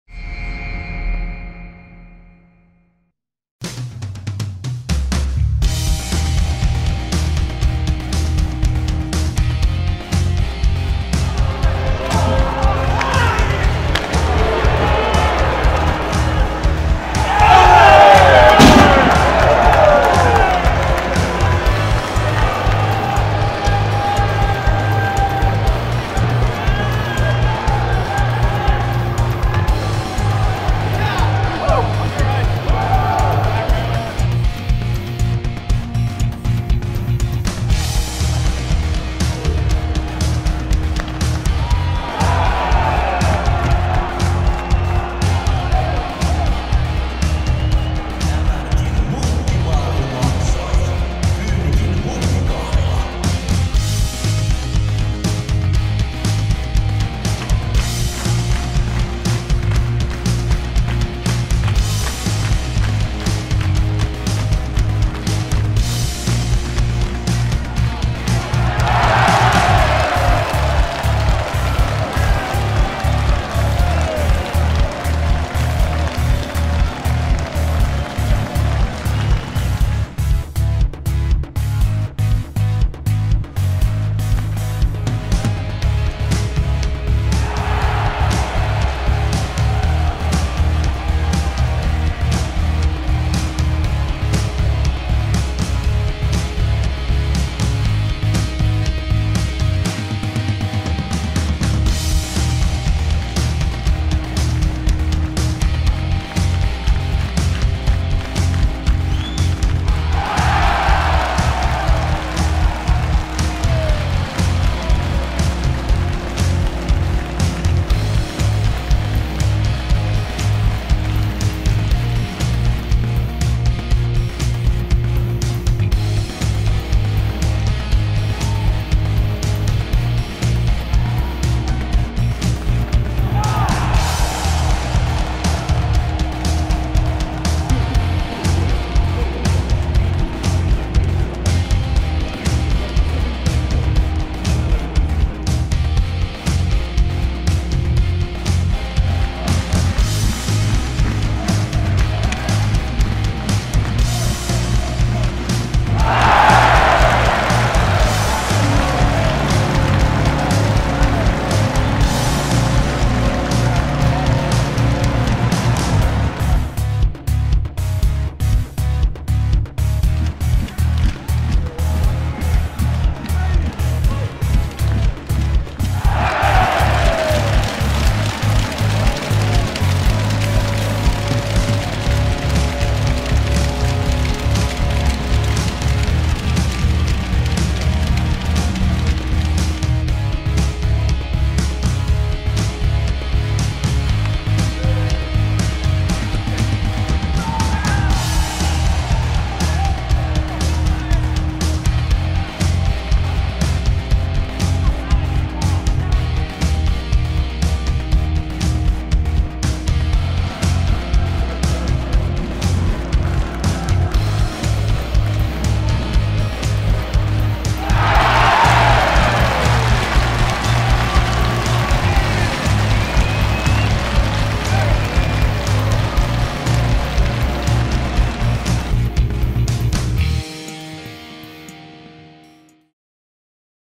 SaiPan runkosarjan 2017-2018 TOP-10 maalit
SaiPan runkosarjan TOP-10 maalit aikajärjestyksessä runkosarjan alusta asti. Näiden videoiden joukosta äänestetään runkosarjan hienoin maali!

Klipit: Ruutu-urheilu